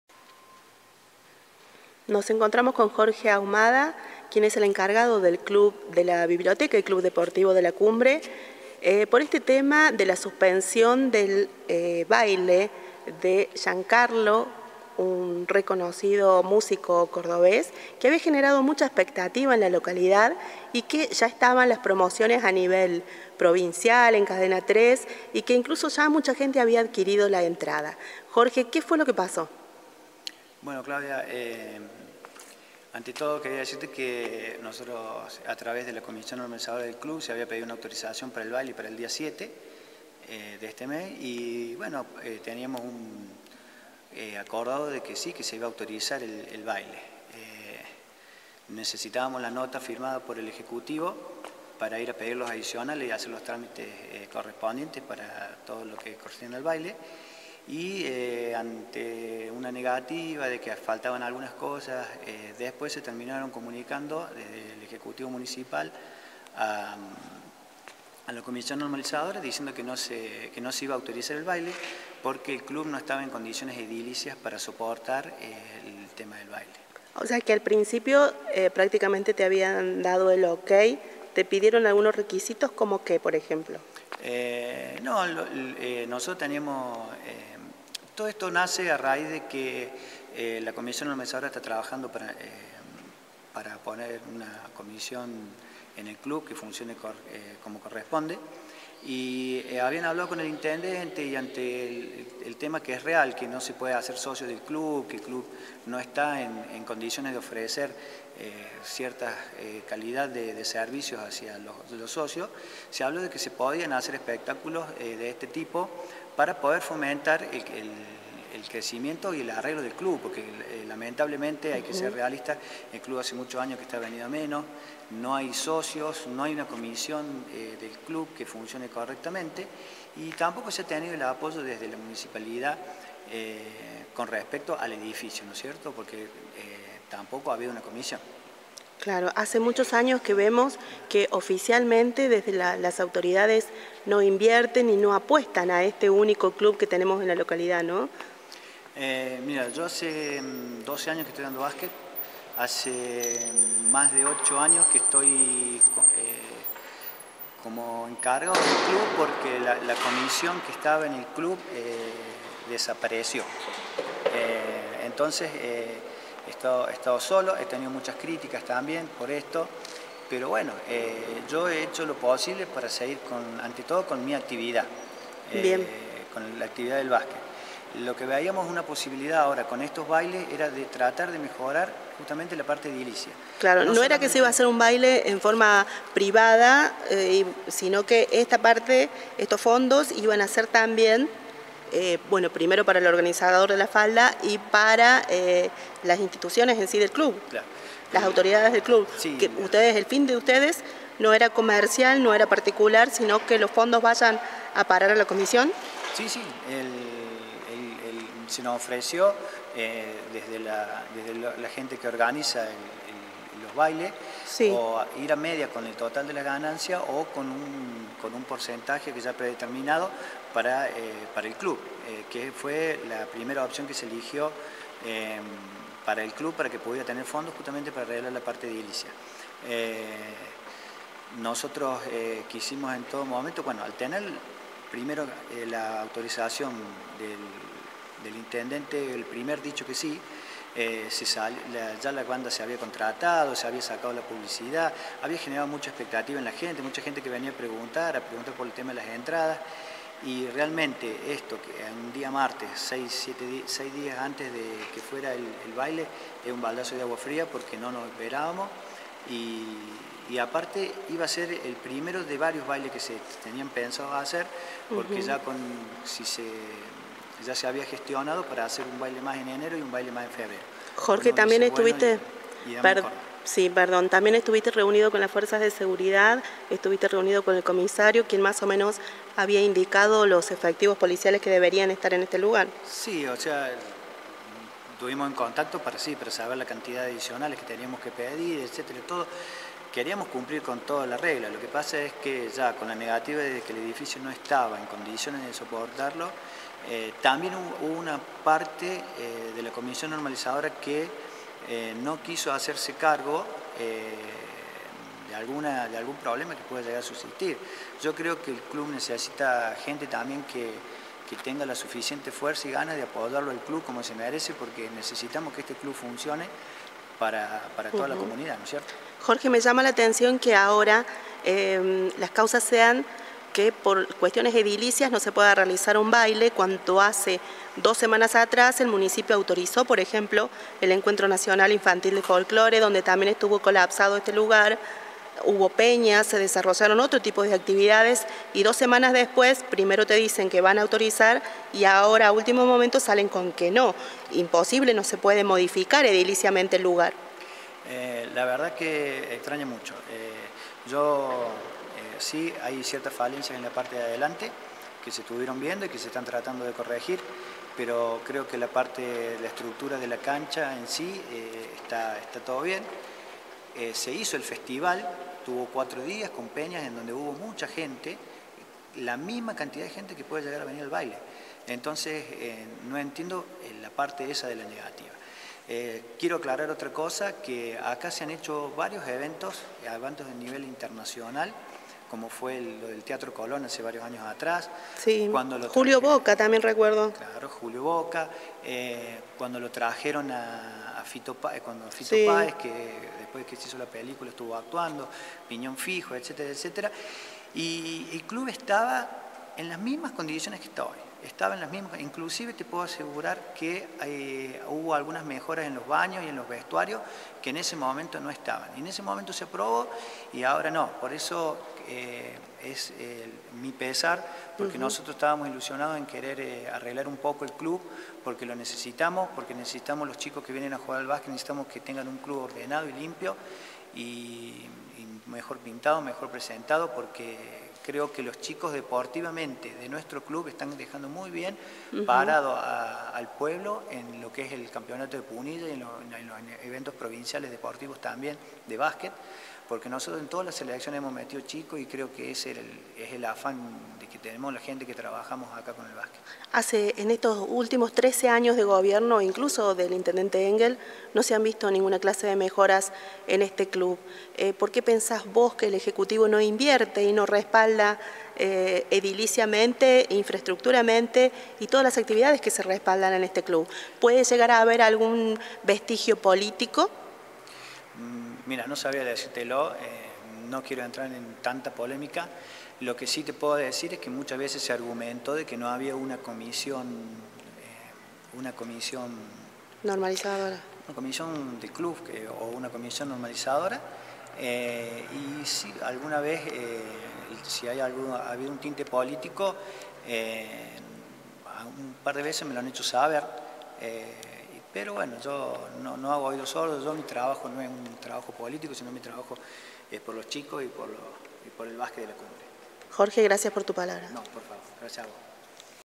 A último momento Engel no autorizó el baile de Jean Carlos en la Biblioteca
¿Será para negros? LA NOMINA entrevistó a Jorge Ahumada, organizador del evento musical popular en la Biblioteca y Club Deportivo La Cumbre quien se mostró molesto por las decisiones de último momento del municipio cuando hace dos semanas atras habian autorizado el Festival de Folclore y peñas por ejemplo. Mirá la nota completa